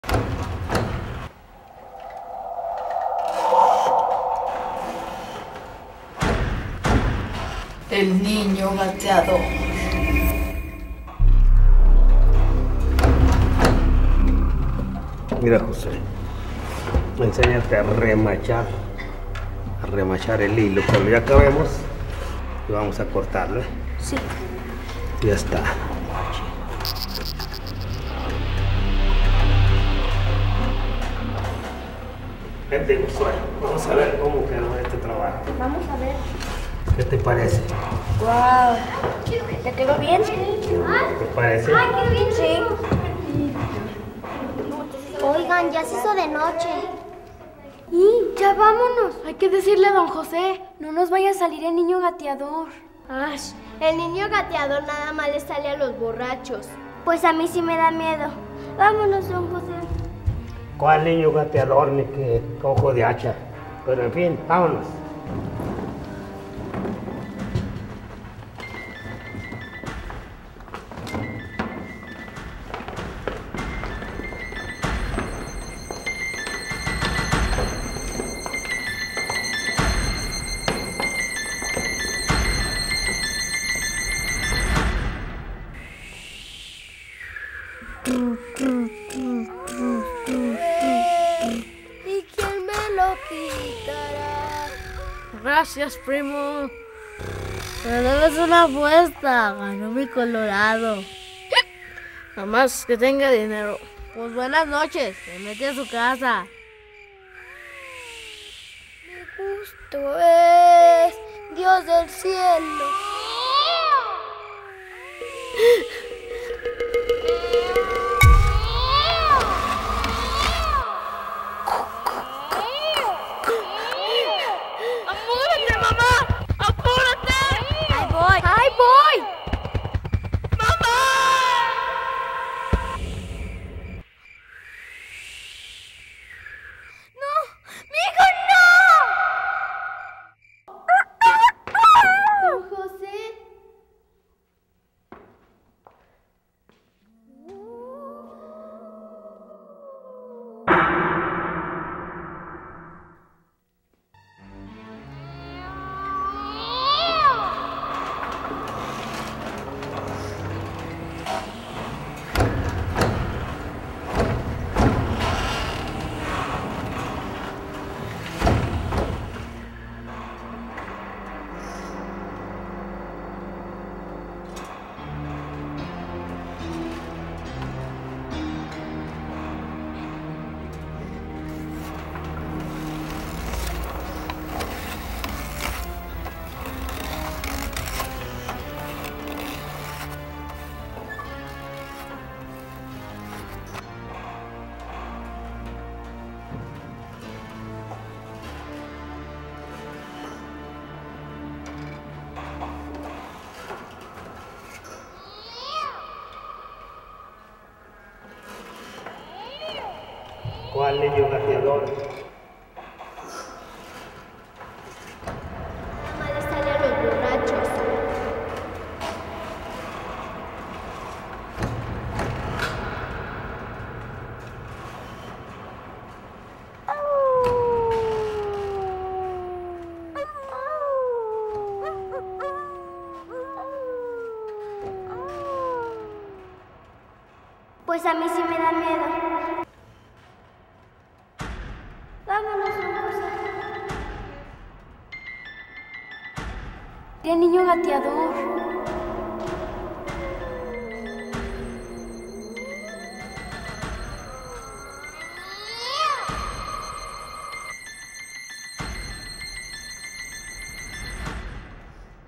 El niño gateador HD
Cuenta la leyenda que "El niño gateador" se aparece, por las noches, en el Taller textil "Cantarranas". Y aunque sólo asusta a los borrachos, una sorpresa les espera a los pequeños tejedores que están aprendiendo a usar el telar.
Video realizado por niñas y niños artesanos de Manitas Tejedoras Villagarcienses. Villa García, Zacatecas.